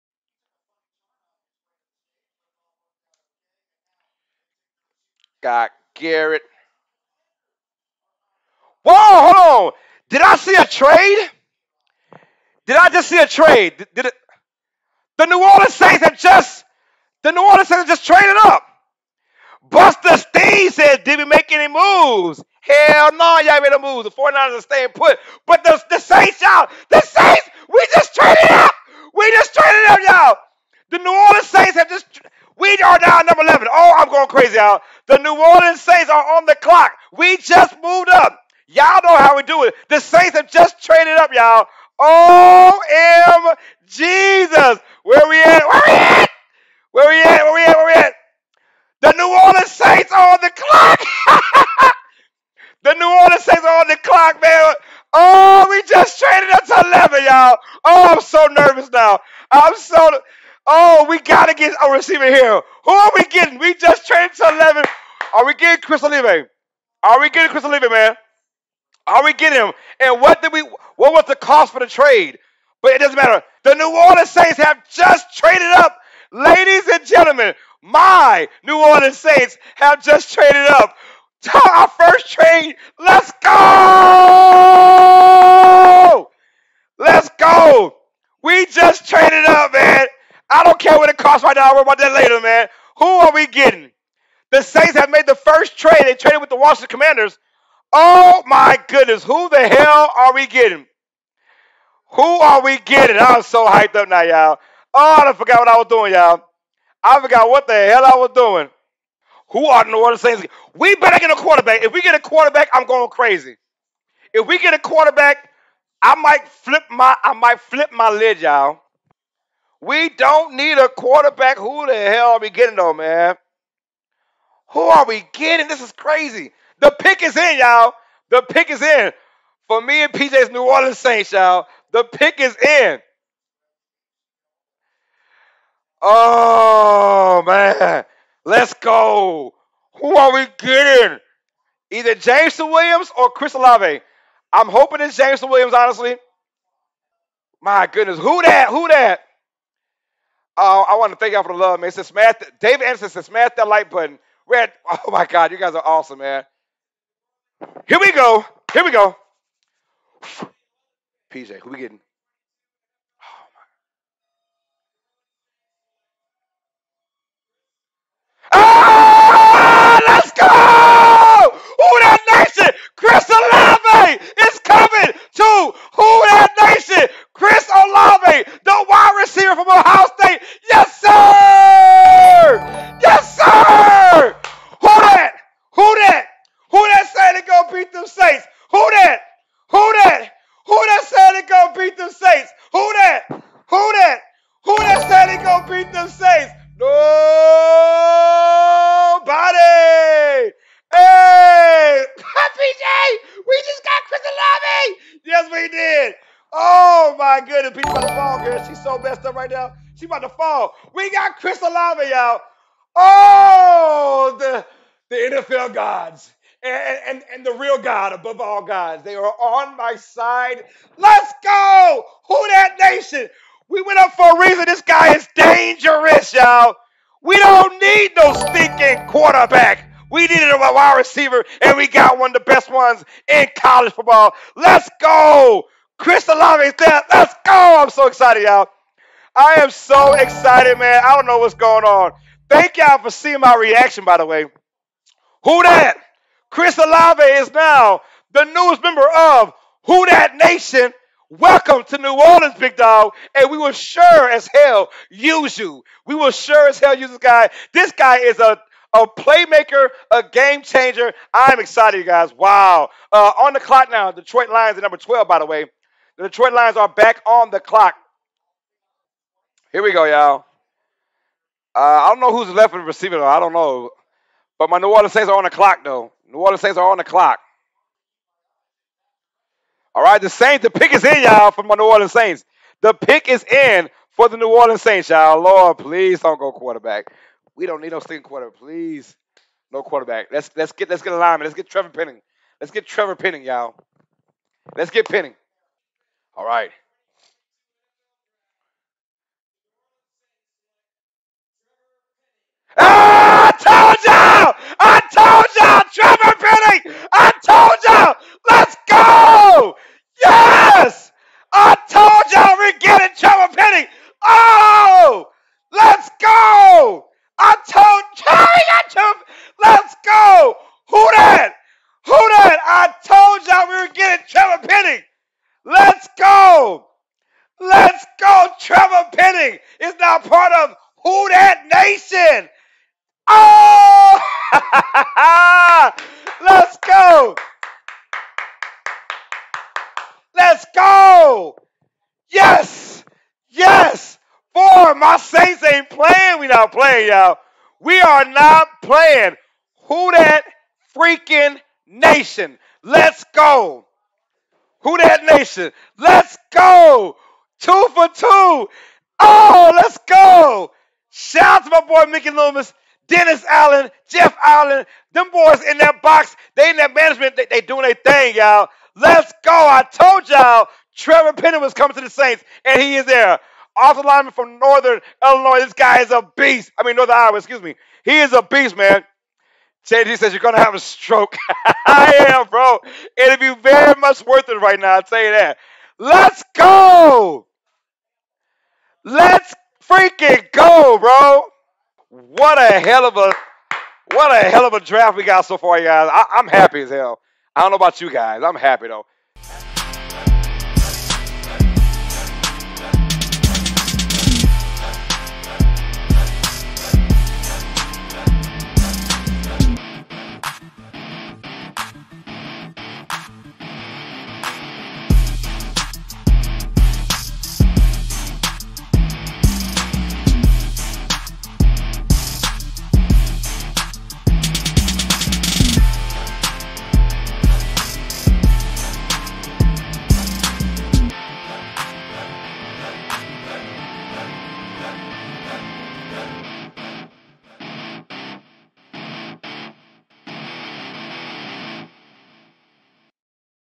A Saints Fan reaction to picking Chris Olave and Trevor Penning
What's up Sports fans! Check out my reactions of my New Orleans Saints two first round picks in getting Chris Olave and Trevor Penning!

Stay Smart. Stay UNeducated